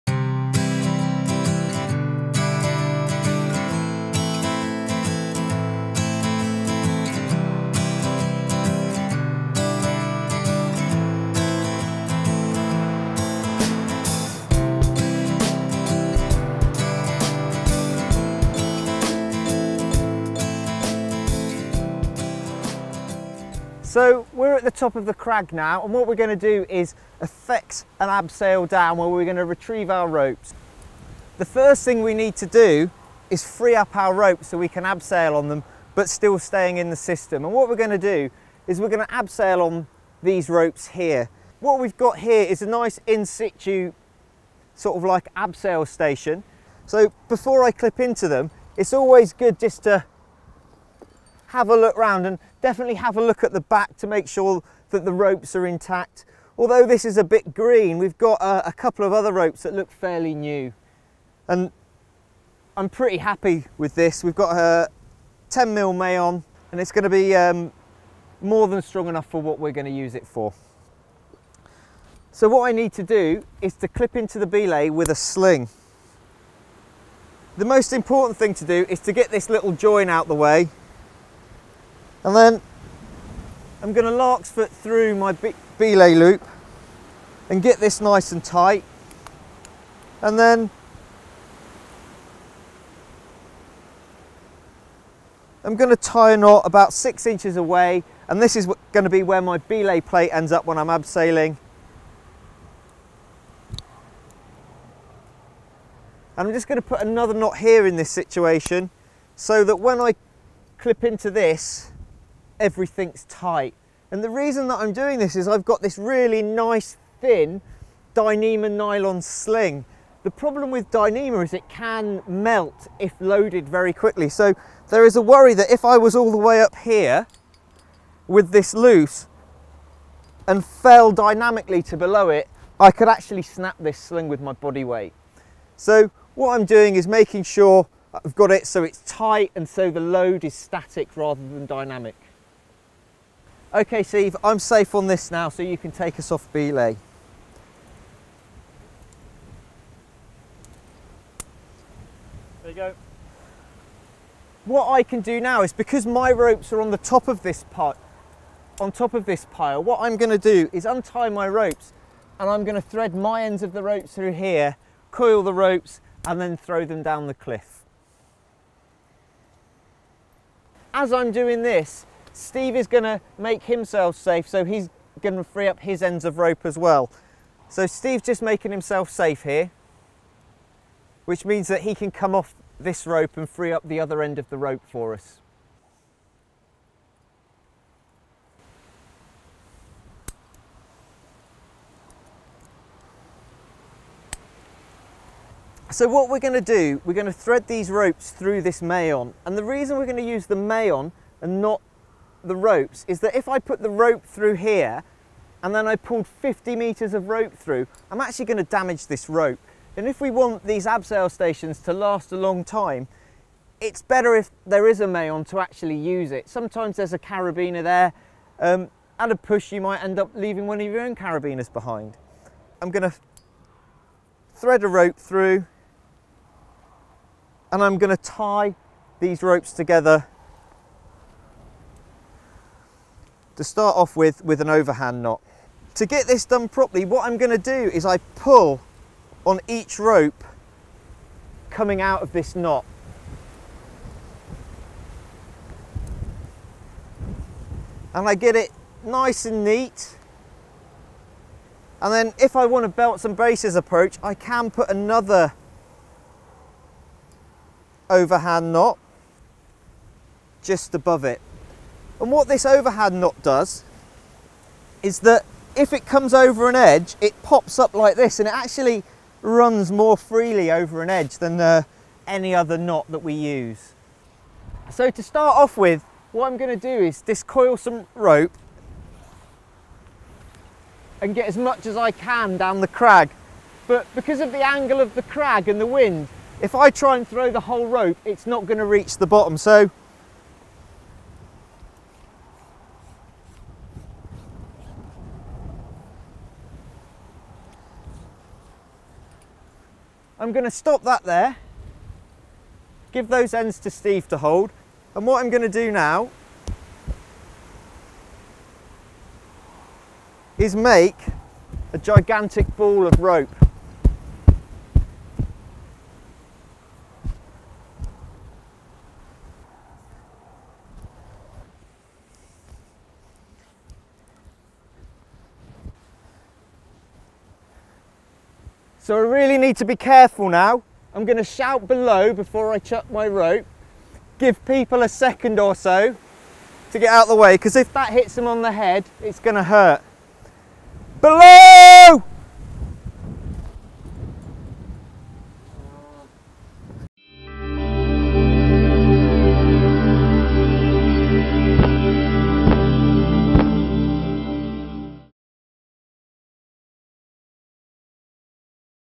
How to: multiple abseils part one
How to make multiple abseils - part one.

Multi-pitch trad climbing skills. A series of short films produced exclusively for BMC TV in association with AMI (the Association of Mountaineering Instructors) and DMM

We're the BMC. Climb walls, rocks, hills, ice or mountains?